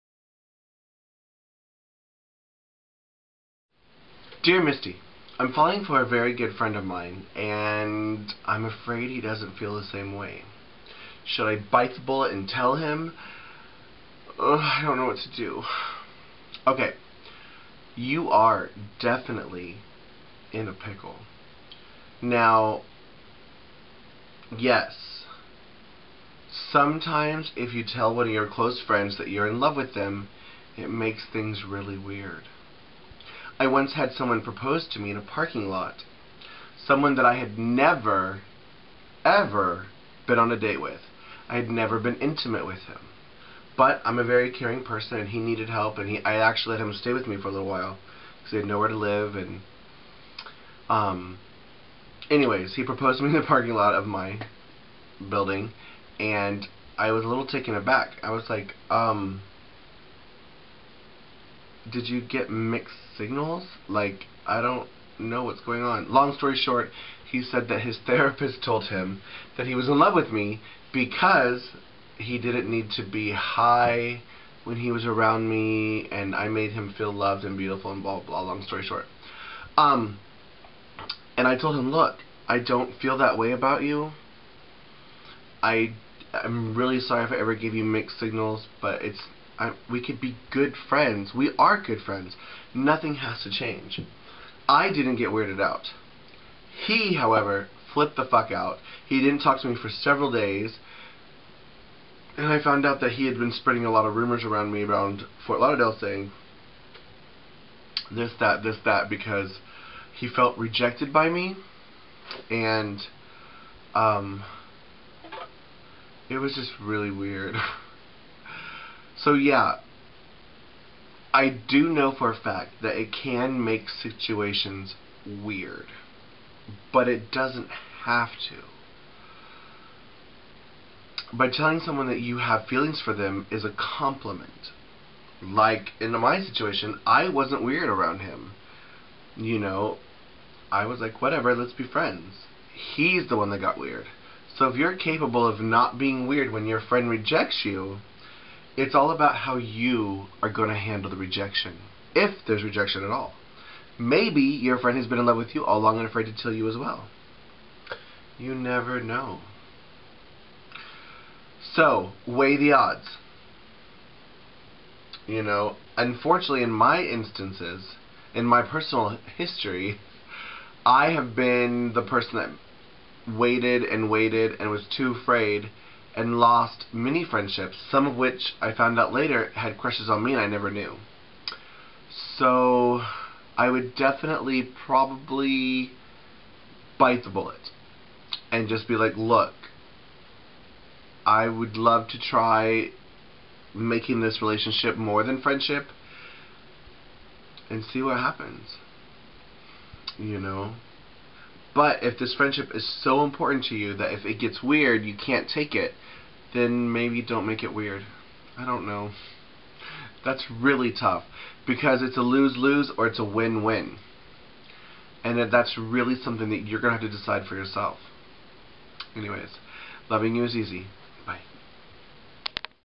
Ask Misty - I have a crush on a Close Friend
Dear Misty, hey gurl it's Nat again, i think i'm falling for a real close friend but am afraid telling him how i feel will make things weird between us if he doesn't feel the same about me. Should I bite the bullet & tell him anyway?     from queensfan79

I think that MOST OFTEN as humans we are attracted to others for whatever reason and if something romantic doesn't become of it... many times we become friends with them. so I think its common that the sub-line of many friendships is a crush on one end or the other. (unless you share something in common like a job hobby etc) but click here for my answer to your question :) XOXO

(you are on my youtube channel but here are my other networks)